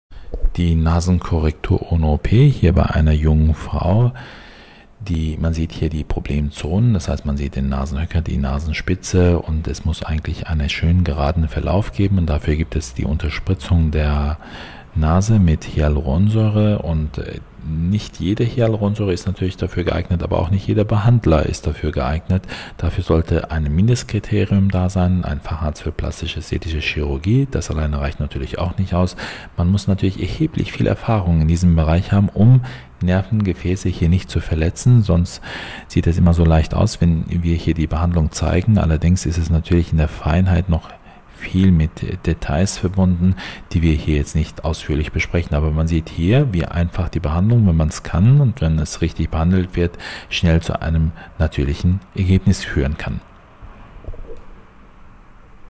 Mc Aesthetics präsentiert die Nasenkorrektur
Nala W. aus München schreibt:,,Ich habe meine Wunschnase.
Ich bin sehr zufrieden mit meiner Entscheidung meine Nase bei Mc Aesthetics machen zu lassen. Ich kann mein Glück nicht fassen. Die Beratung war schon sehr positiv und stimme mich ein. Die Behandlung war sehr gut auszuhalten und ich komme in zwei Wochen auf jeden Fall wieder.